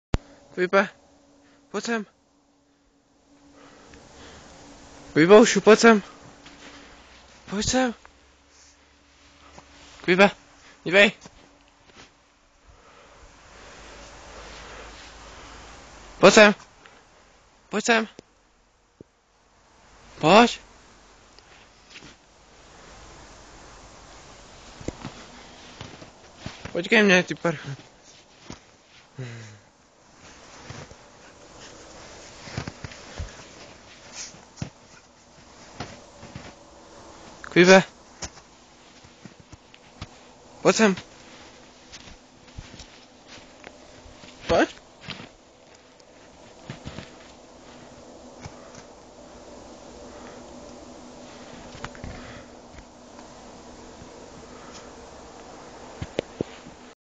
Kvip ve sněhu
Na zahradě tehdá jak bylo sněhu víc než dost